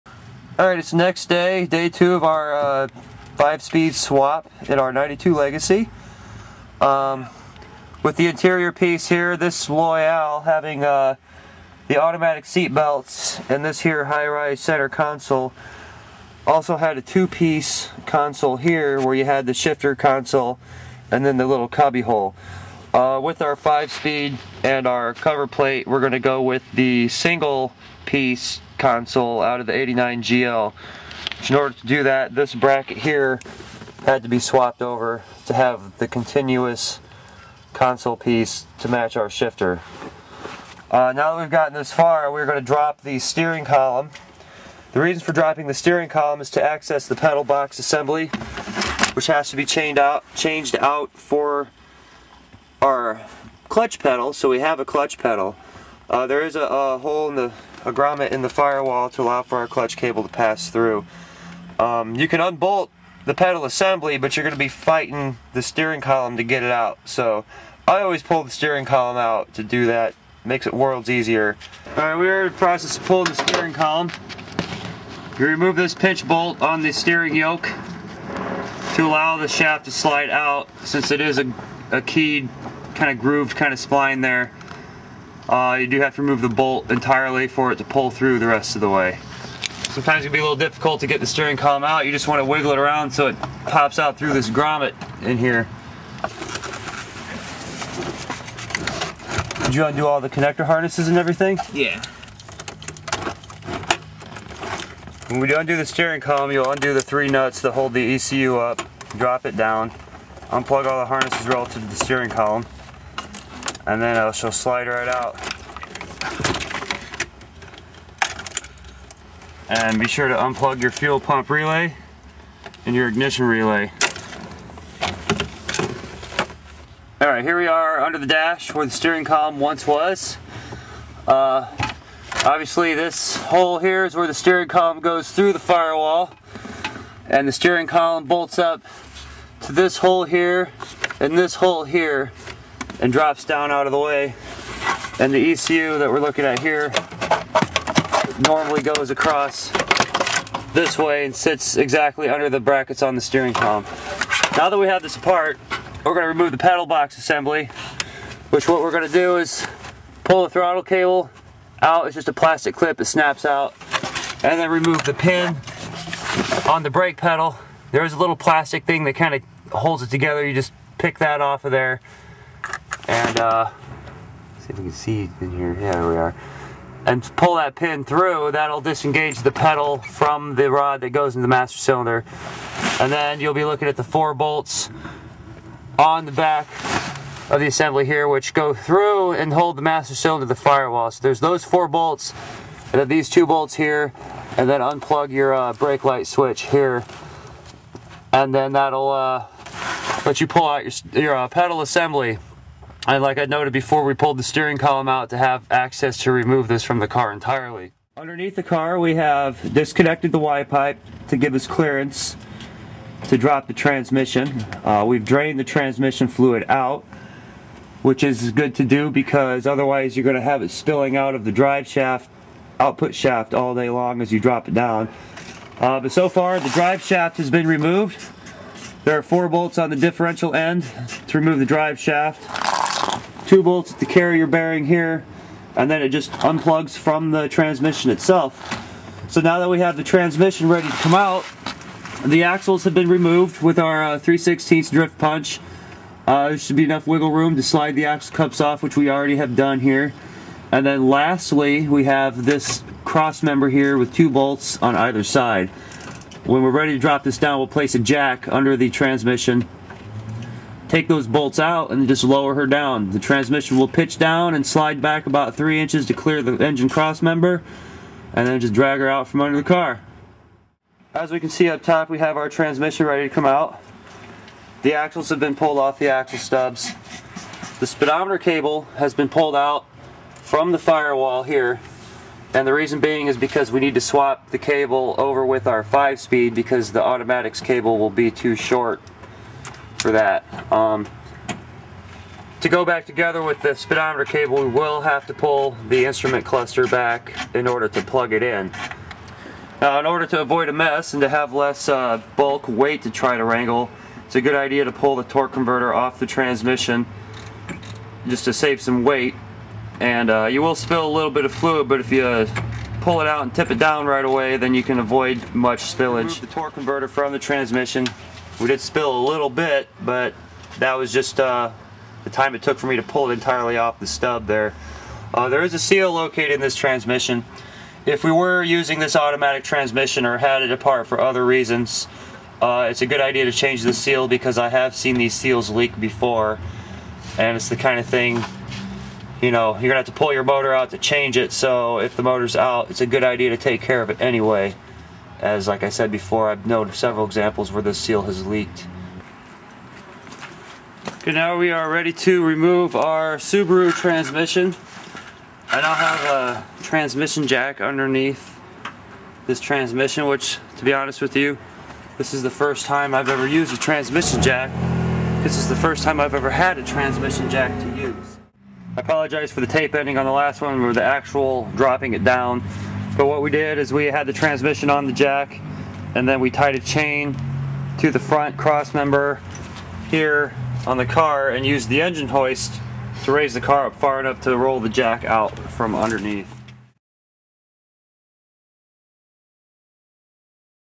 Subaru AT to 5MT Swap Pt. 1/2
The Art of Subaru Maintenance #16
EA82 Automatic Transmission Removal